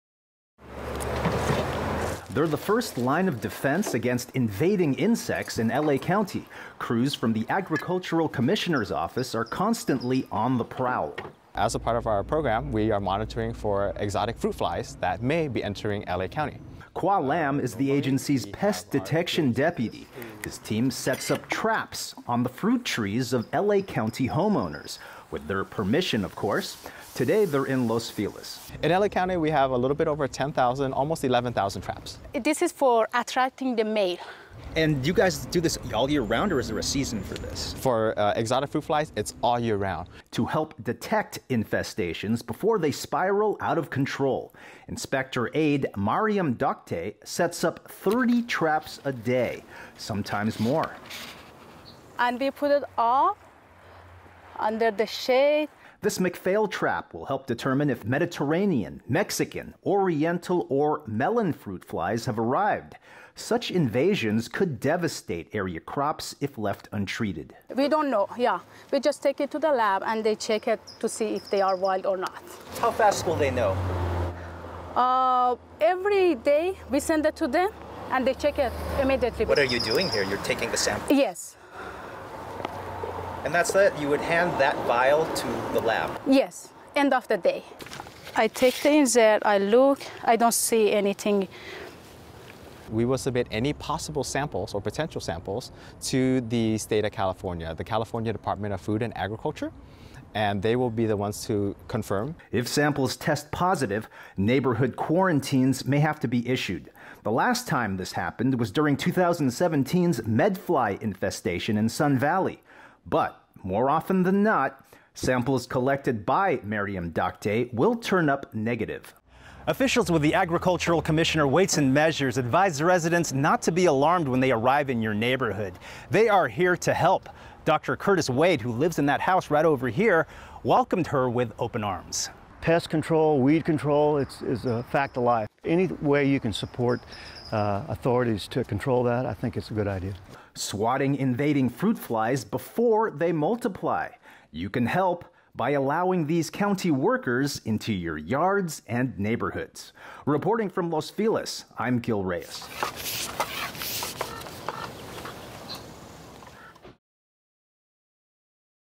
Pest swat team on patrol
It's the season of invading fruit flies, and crews from the Agricultural Commission are launching an offensive by placing traps to test specimens. Without proper abatement, the pests can destroy area crops and create headaches for homeowners.